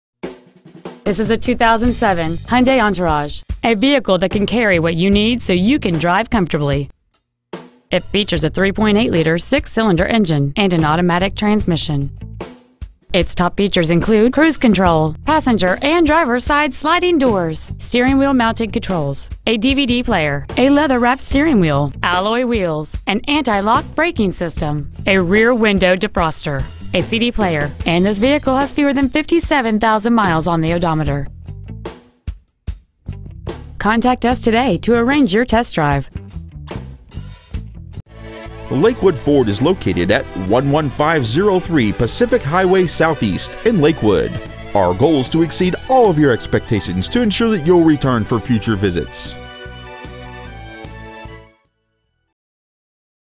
2007 Hyundai Entourage Tacoma WA
We are proud to present this 2007 Hyundai Entourage .

 Year : 2007

 Make : Hyundai

 Model : Entourage

 Engine : 3.8L SOHC V6

 Trans . : Automatic

 Exterior : Blue

 Miles : 56,355

 Interior : Gray

 Lakewood Ford

 11503 Pacific Highway SE

 Preowned 2007 Hyundai Entourage Lakewood WA